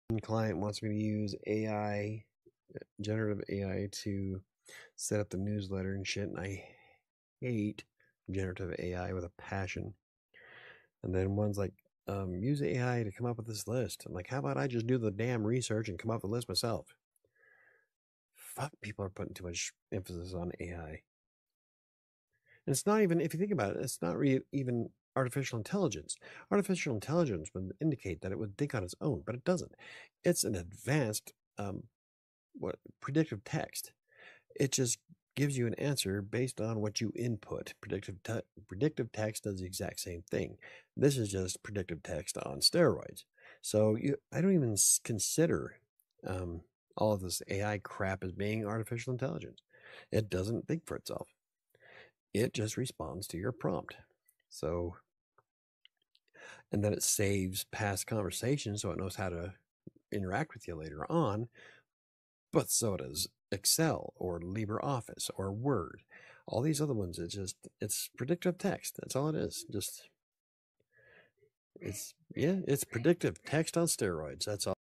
Generative AI: Just Predictive Text on Steroids?
So many similarities between AI and predictive text.